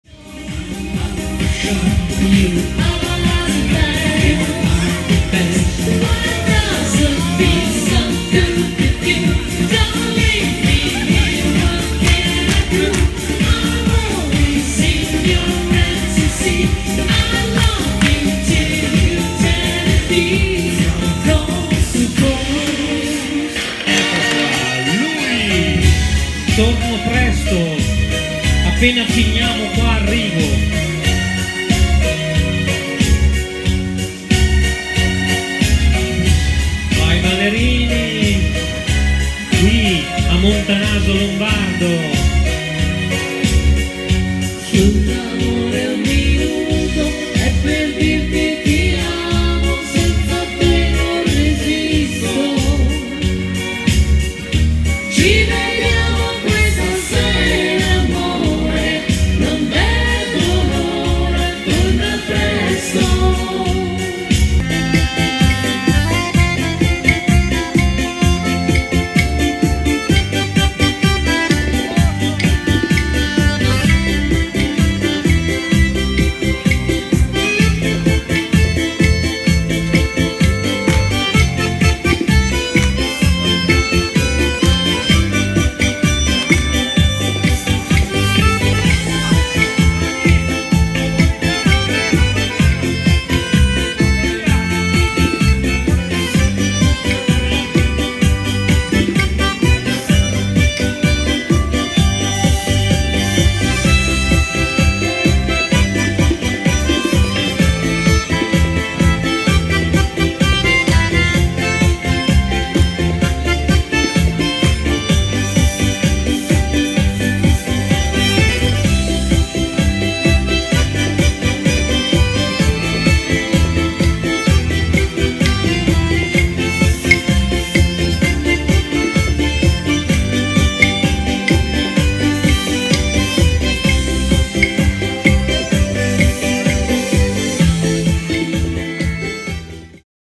Orchestra Daniele Cinquanta - Festa Pro Loco Montanaso Lombardo - 16/07/2022
Grazie agli amici della Pro Loco di Montanaso Lombardo che anche quest'anno ci ha voluti su questo splendido palco!